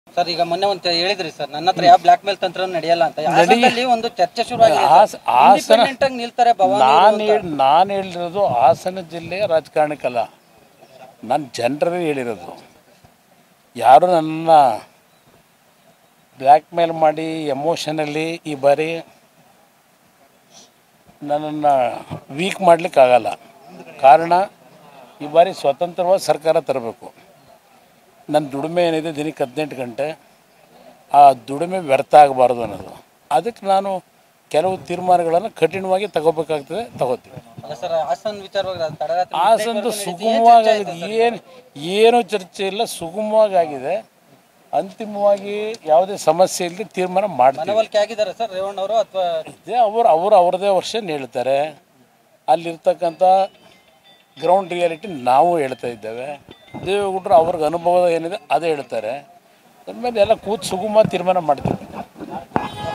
ನಾನು ಬ್ಲಾಕ್​​ಮೇಲ್​​ಗೆ ಬಗ್ಗೋದೇ ಇಲ್ಲ.. ನನ್ನನ್ನು ವೀಕ್​ ಮಾಡಲು ಆಗೋದೇ ಇಲ್ಲ - ಮಾಜಿ ಸಿಎಂ ಹೆಚ್.ಡಿ ಕುಮಾರಸ್ವಾಮಿ
Btv News Kannada : - ನಾನು ಬ್ಲಾಕ್​​ಮೇಲ್​​ಗೆ ಬಗ್ಗೋದೇ ಇಲ್ಲ.. ನನ್ನನ್ನು ವೀಕ್​ ಮಾಡಲು ಆಗೋದೇ ಇಲ್ಲ - ಮಾಜಿ ಸಿಎಂ ಹೆಚ್.ಡಿ ಕುಮಾರಸ್ವಾಮಿ